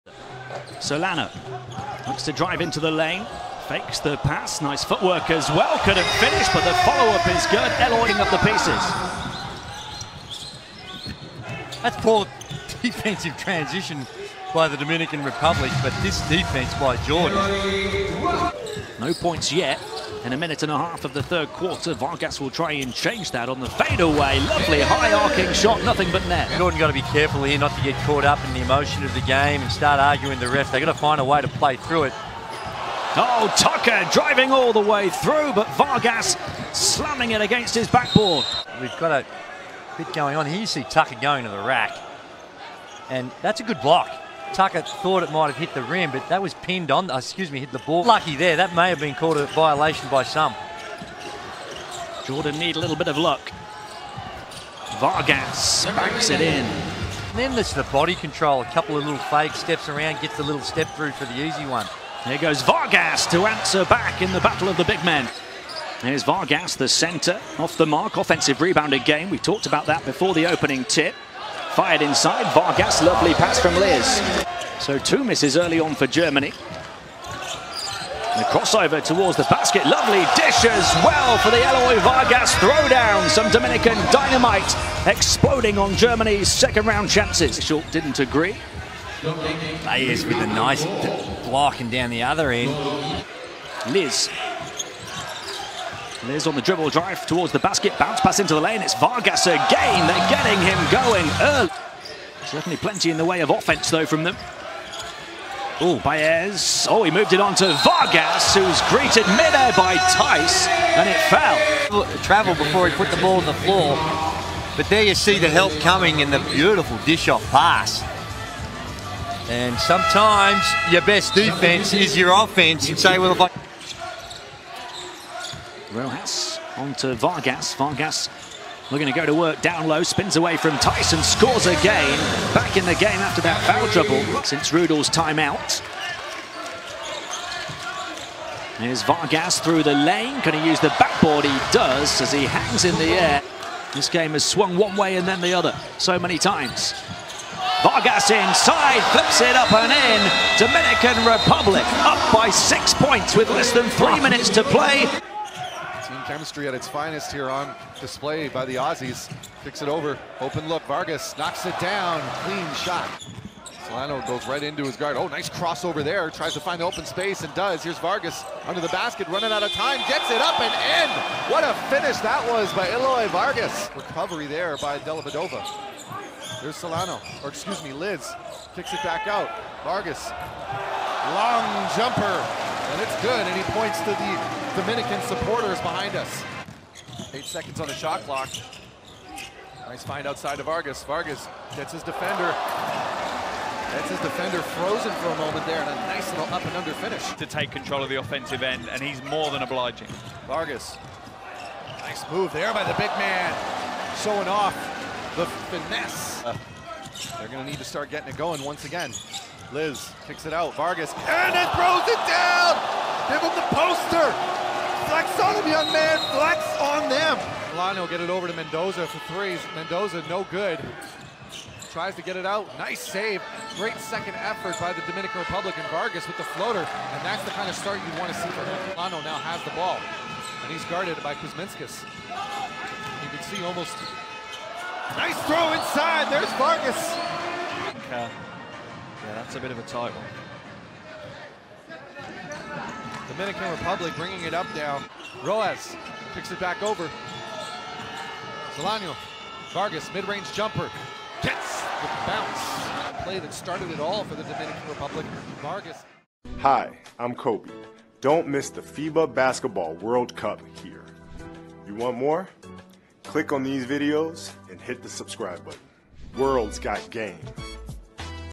Eloy Vargas - ALL his BUCKETS & HIGHLIGHTS from the FIBA Basketball World Cup 2019
Enjoy this compilation of every single field goal & highlight from Dominican Republic's Eloy Vargas during his run at the FIBA Basketball World Cup 2019.